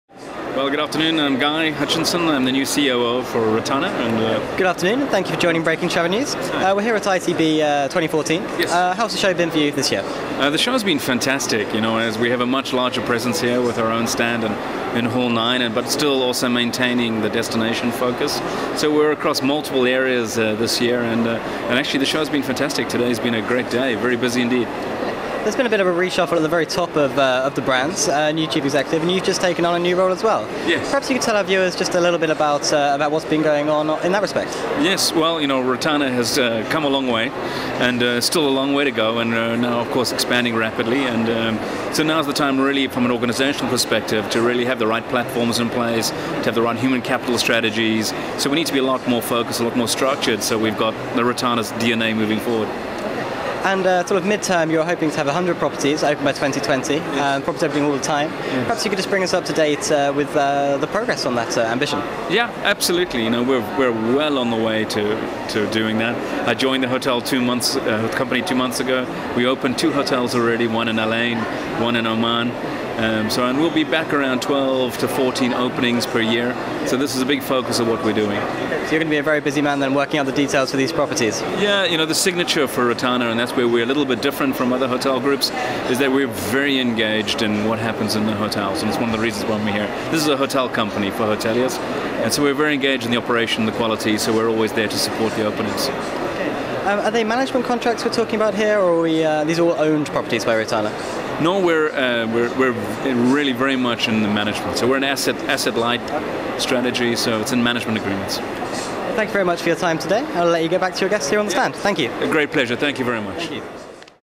Guy Hutchinson, COO, Rotana Hotels
Guy Hutchinson, COO, Rotana Hotels, talks to Breaking Travel News on Day 2, at ITB 2014.